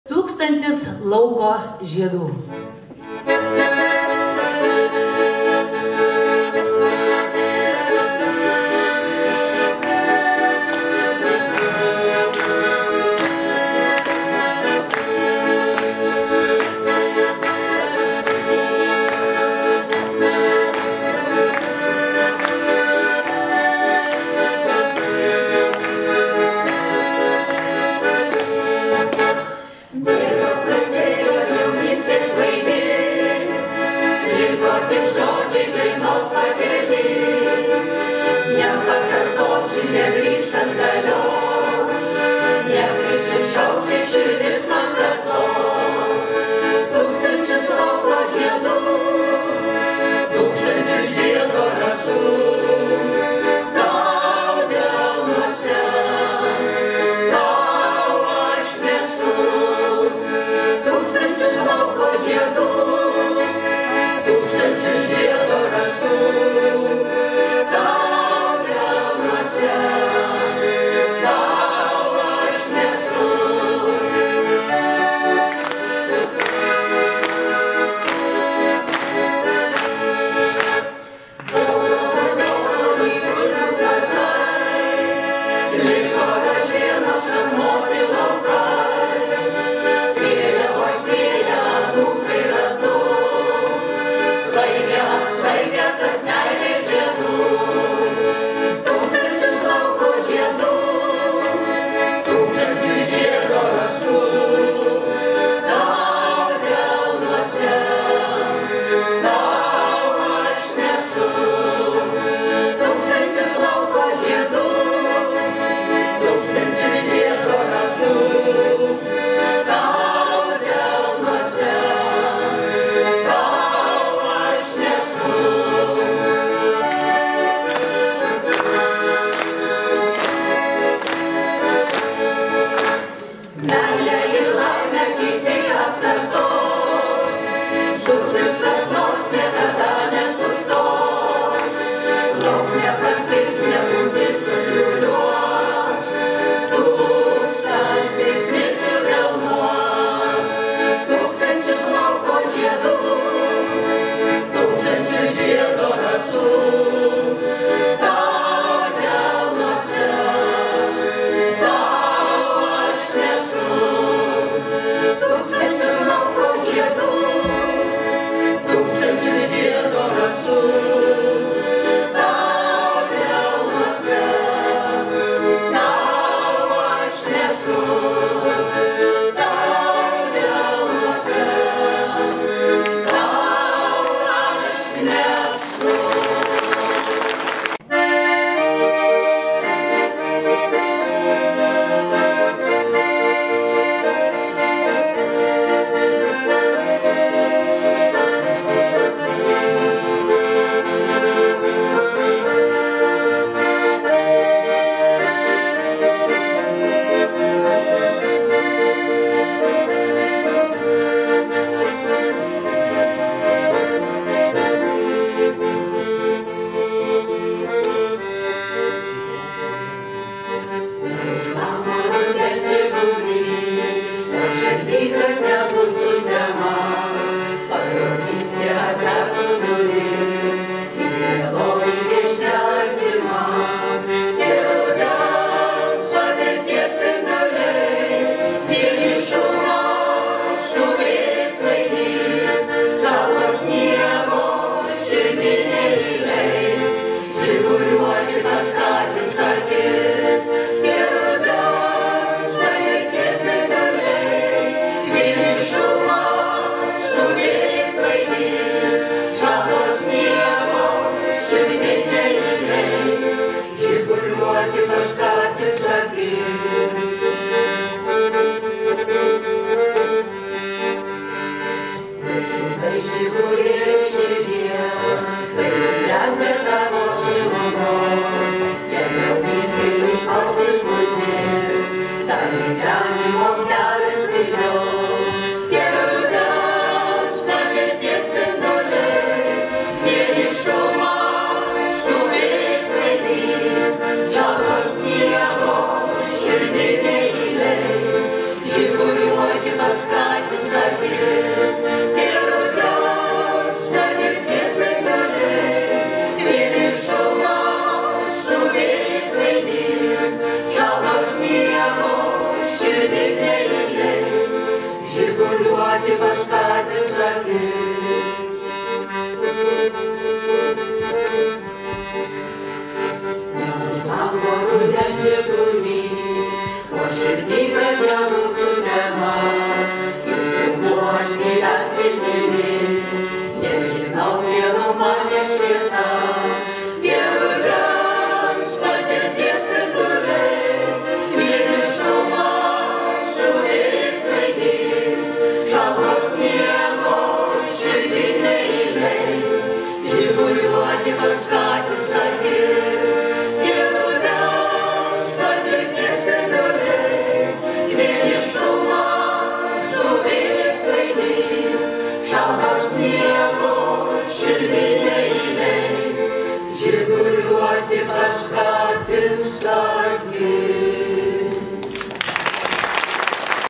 ''JOTVAINIAI'' 2 DAINOS 2022 11 05 Alytus
Renginuko ''DRAUGIŠKAS RUDUO'' puikaus kolektyvo ''JOTVAINIAI'' DAR 2 DAINOS.
Visų grupių trumpa prezentacija yra šioje nuorodoje žemiau

NEGAILĖKIME LAIKŲ IR ...PRENUMERUOJAMĖS KANALĄ!